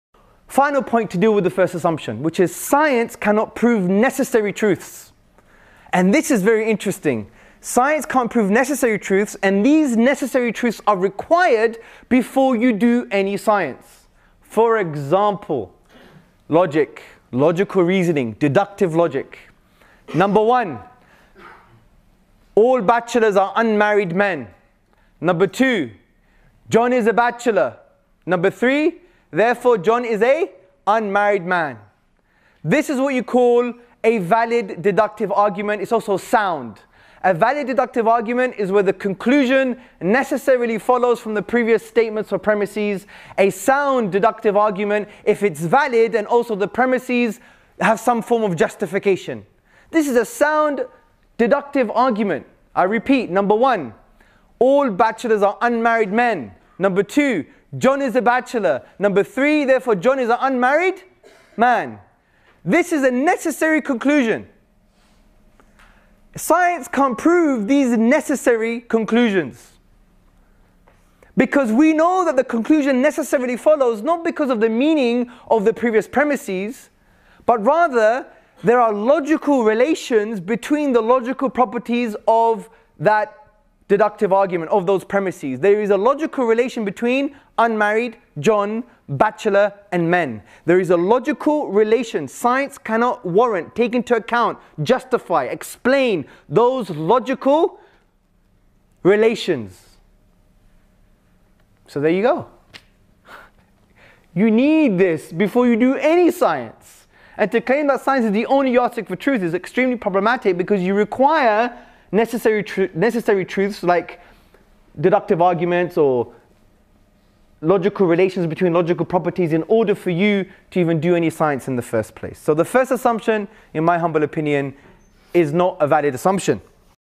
Can Science prove Neccessary Truth ? By ustadh Hamza Andreas Tzortzis
Is science the only way to truth?  Can science prove everything? What other way can we prove something to be true ?